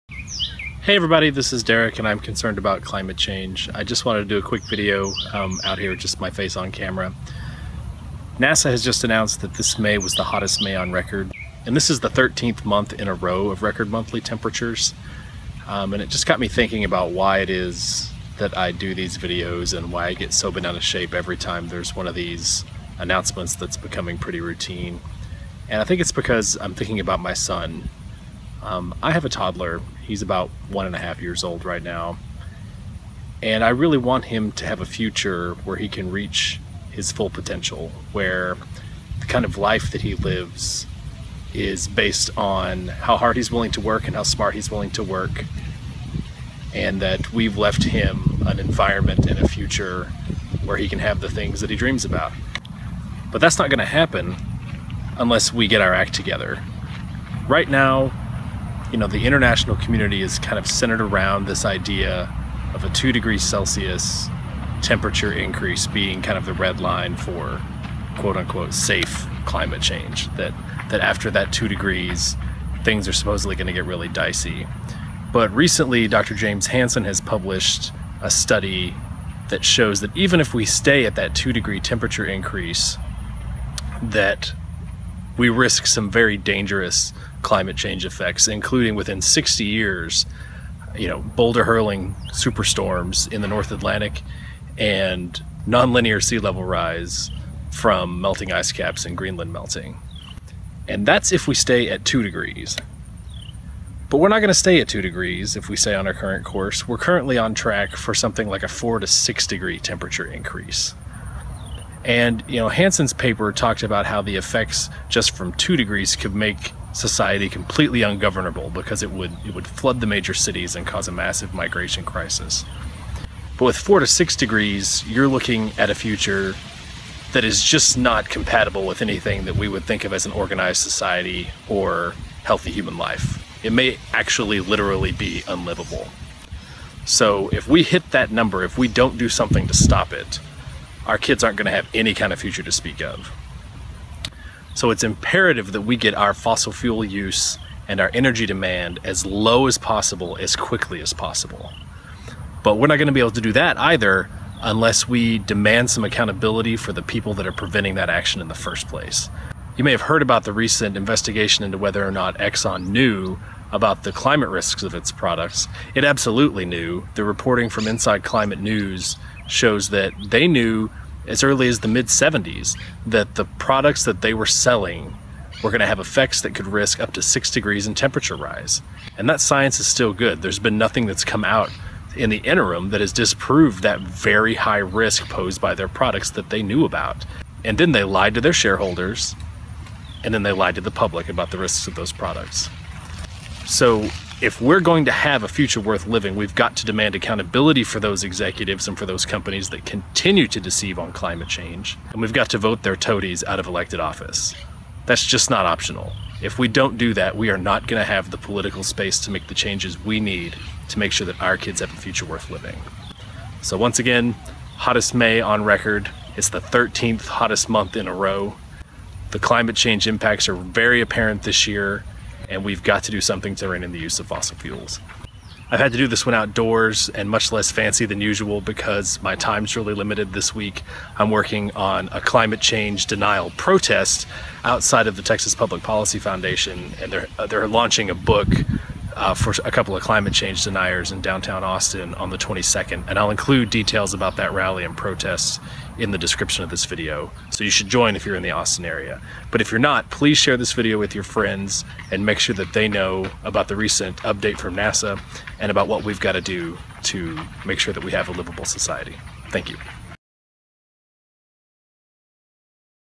Climate Change: NASA says May 2016 was hottest May on record
Experimenting with Adobe Clip on my phone...bear with me!

NASA just announced that May 2016 was the hottest May on record. We're continuing to blow pasts temperature records on our way to 2°C and beyond.

If you're in the Austin area, join us to fight back against climate change denial